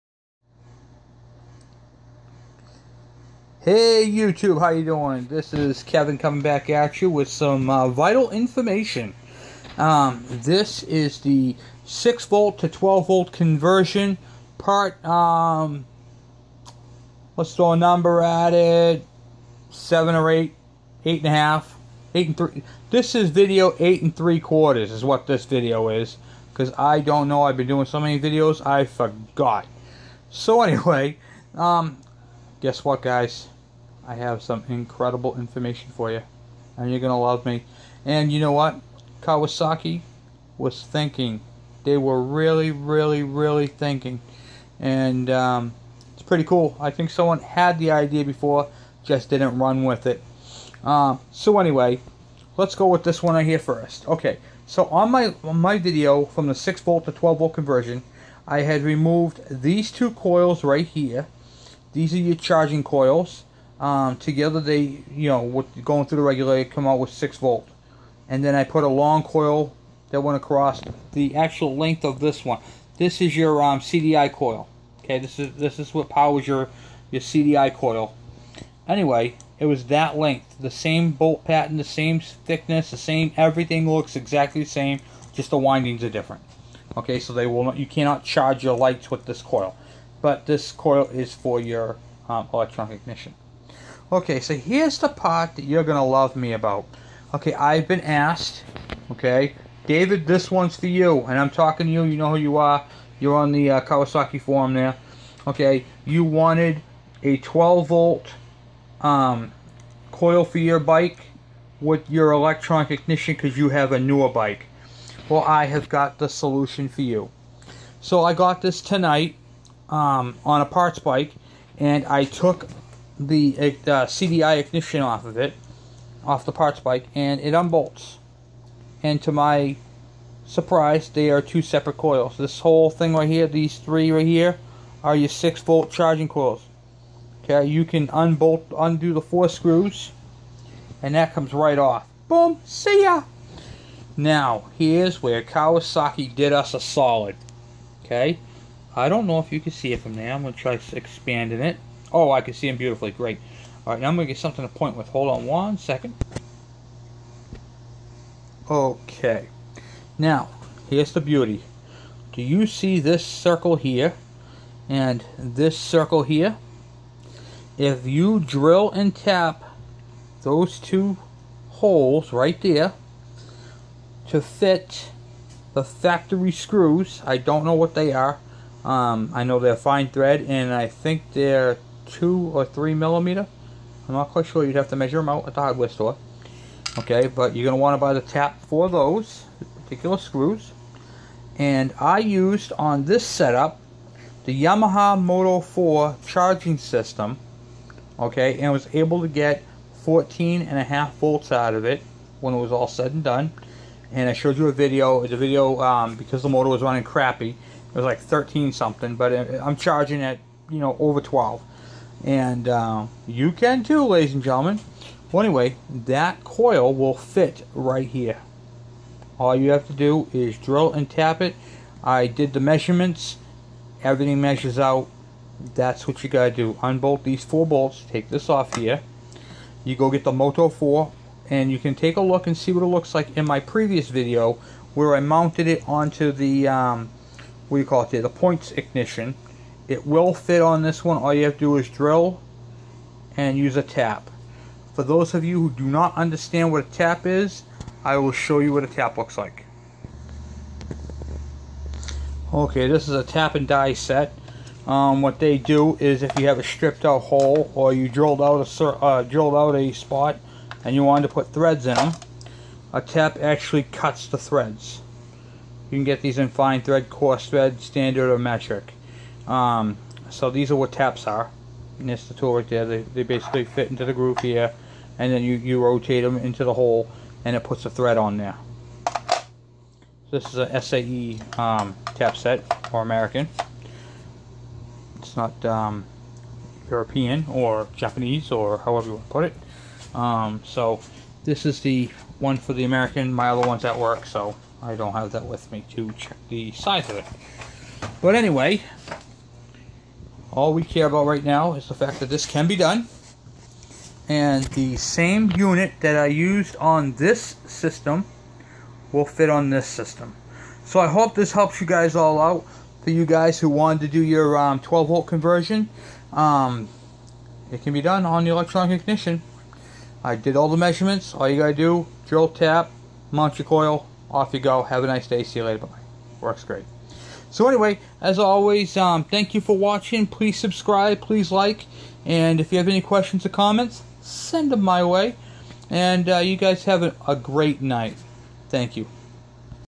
KE100 CDI PLUS 6 volt to 12 volt conversion part 8 3/4 lol
This video is an update to a video 6 volt to 12 volt  conversion this update is for use with CDI